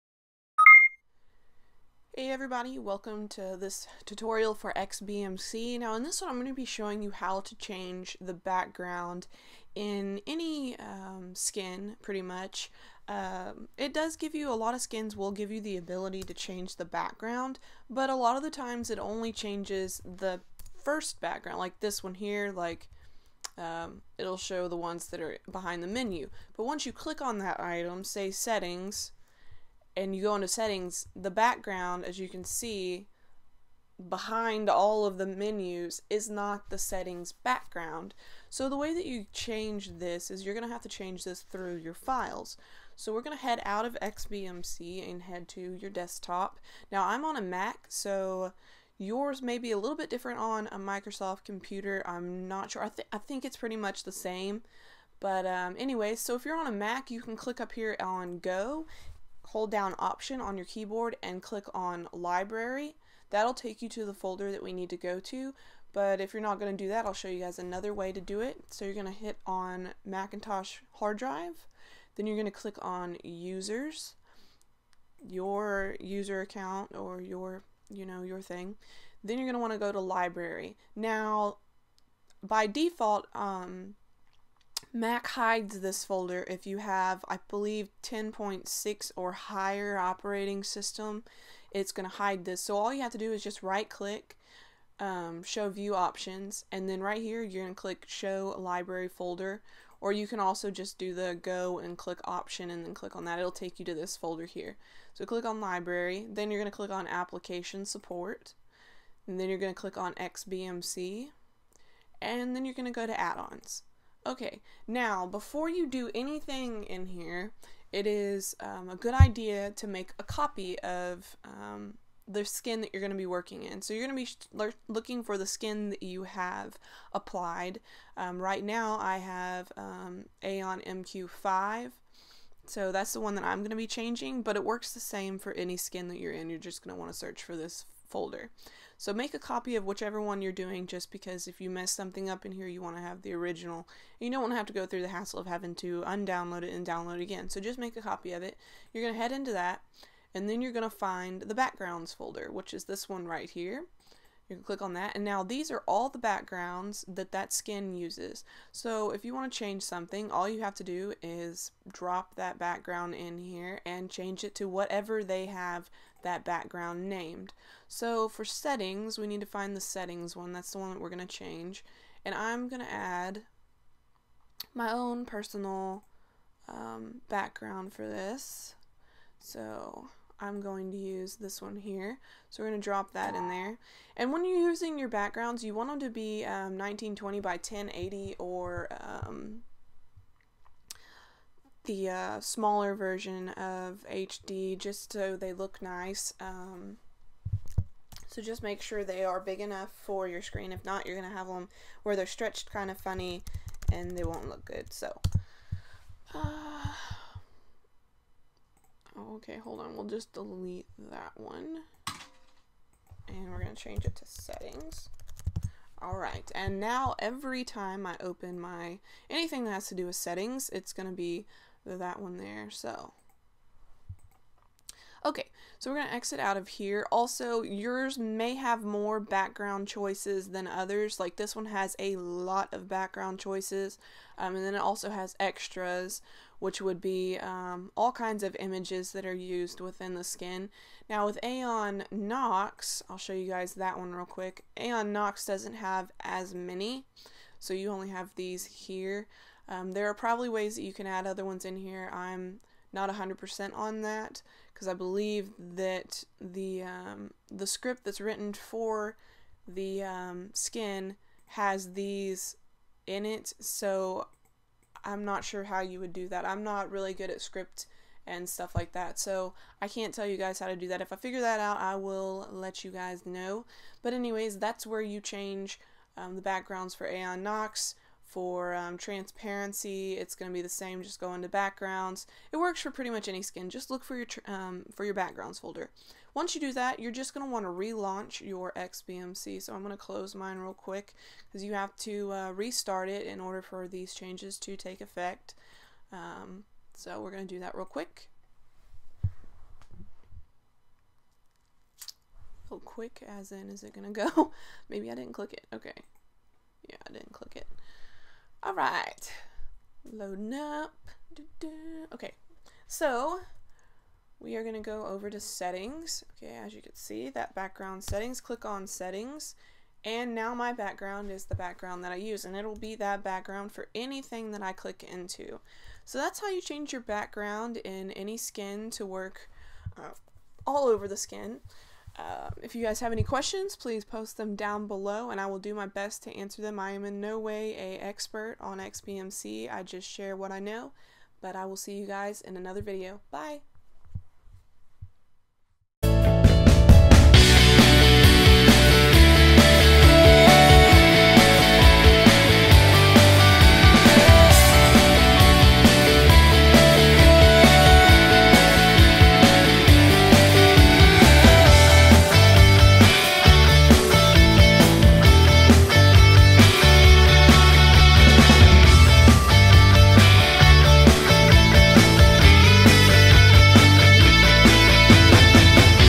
XBMC Tutorial: How To Change Any Background Through Files
In this XBMC tutorial I show how to change any skin's background through your files.

Locations of files on different operating systems
Android
Android/data/org.xbmc.xbmc/files/.xbmc.userdata/
OR sdcard/Android/data/org.xbmc.xbmc/files/userdata/

iOS
/private/var/mobile/Library/Preferences/XBMC/userdata/

Linux
~/.xbmc/userdata/

/Users/YourUserName/Library/Application Supports/XBMC/userdata/

OpenELEC
/storgare/.xbmc/userdata/

Windows
Users\YourUserName\AppData\Roaming\XBMC\userdata\